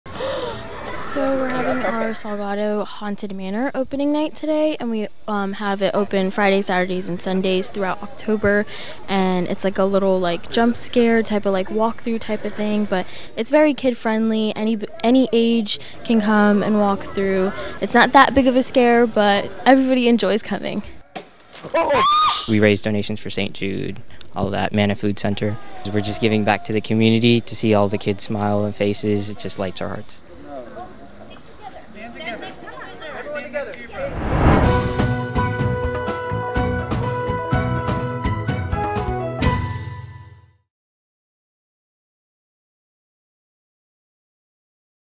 Halloween Adventure Awaits at 'Salgado's Haunted Manor' in Germantown
A Germantown family is inviting the community to experience a Halloween-themed "Salgado's Haunted Manor" throughout October. The free haunted trail offers a scary Halloween experience featuring fog machines, strobe lights, animatronics, eerie sound effects, and more.